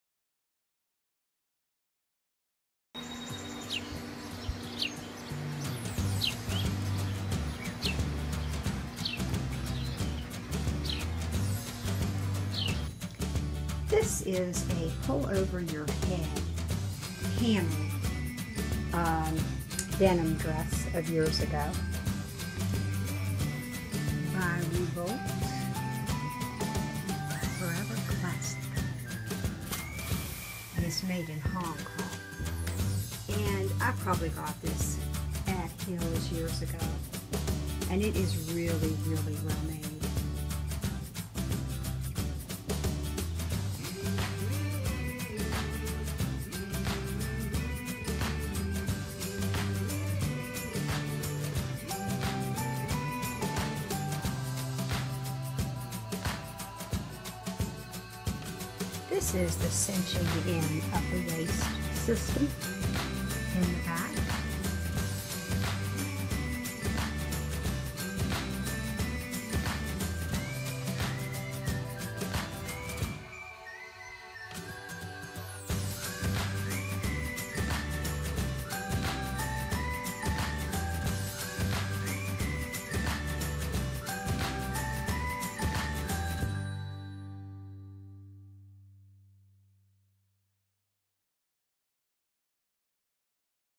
Using my old Clothes with hightop sneakers
Can't stop wearing my old clothes. The quality of my old clothes are superior to the quality of clothes of today. The background shows the completed leaning fence project.